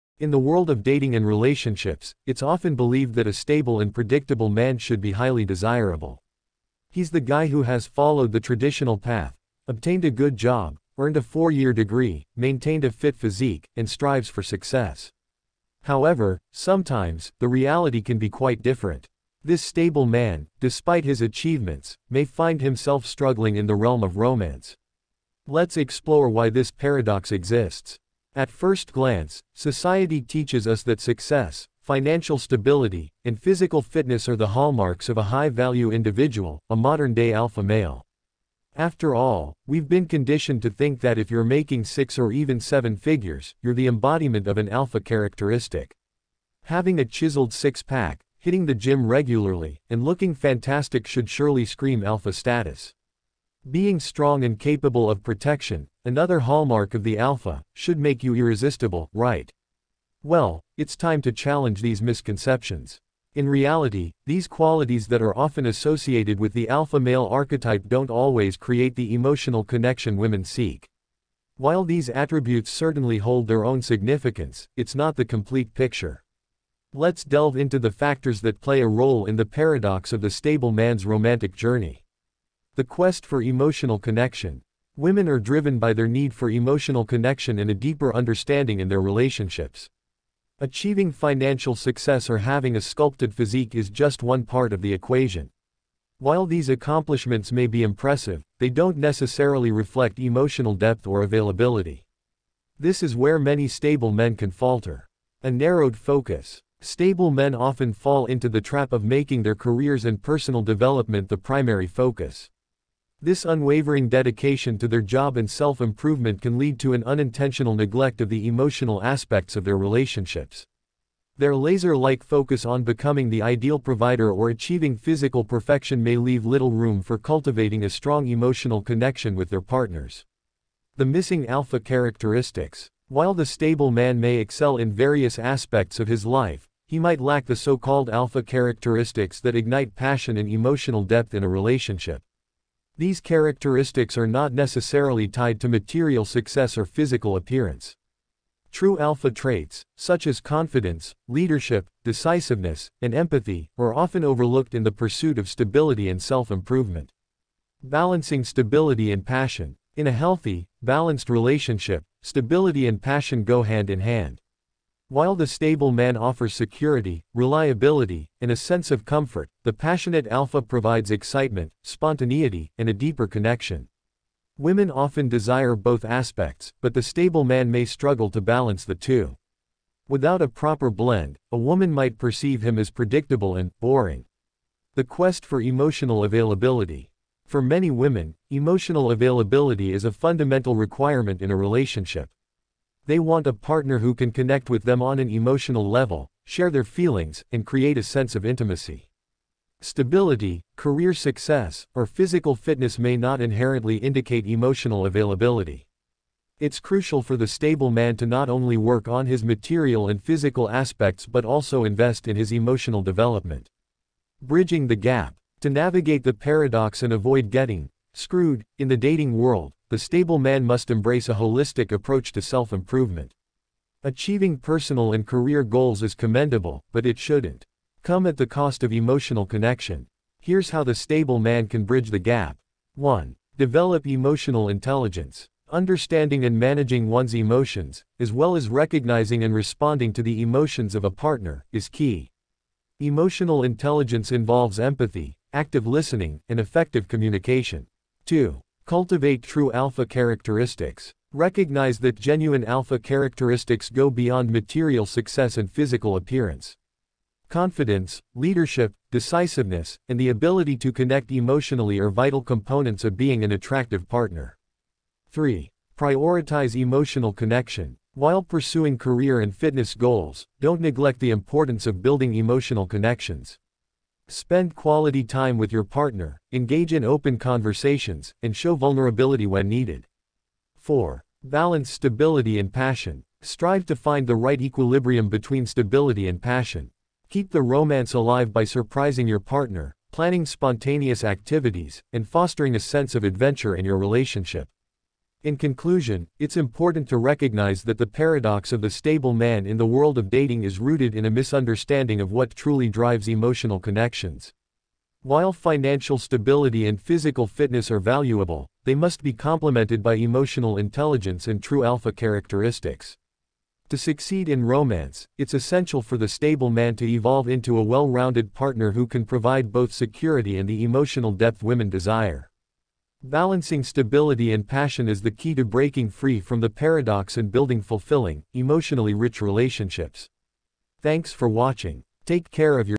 Why WOMEN DIE For JERKS ? Why the Stable Man Can Get Screwed in Love
In the world of dating and relationships, the stable and predictable man is often perceived as a desirable partner. But what happens when this archetype faces challenges in the realm of romance? Join us as we explore the paradox of the stable man in the modern dating landscape.

We'll debunk common misconceptions about alpha characteristics and delve into why financial success and a chiseled physique don't always guarantee emotional connection. Discover why emotional depth is a critical component in forging lasting relationships and how the relentless pursuit of material success can sometimes hinder it.

Learn how to bridge the gap between stability and passion to create fulfilling, emotionally rich relationships. Find out why true alpha traits such as confidence, leadership, and empathy are vital for success in the world of dating. Explore the quest for emotional availability and its significance in building intimate connections.

Join us on this journey to unlock the secrets of becoming an attractive partner by embracing emotional intelligence, prioritizing emotional connections, and balancing stability and passion. It's time to challenge the paradox and pave the way for fulfilling, emotionally satisfying relationships. Don't miss this insightful exploration of the stable man's romantic journey.